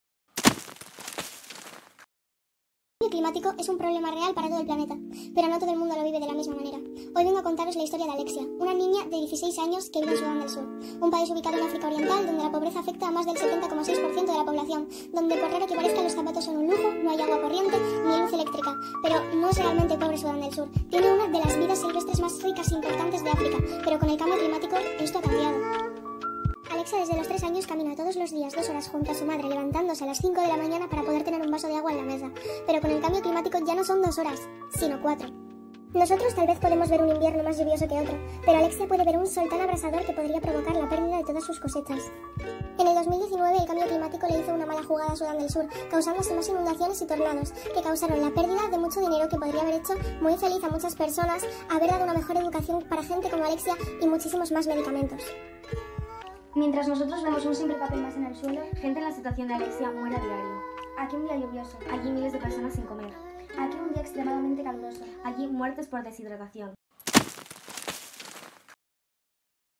Una breve historia
Este clipmetraje relata la vida de Alexia, una joven de 16 años procedente de Sudán del Sur. Se analizan las duras condiciones en las que vive debido a la huella que deja el cambio climático.

Nuestra Señora del Rosario 4ESO
Festival de Clipmetrajes de Manos Unidas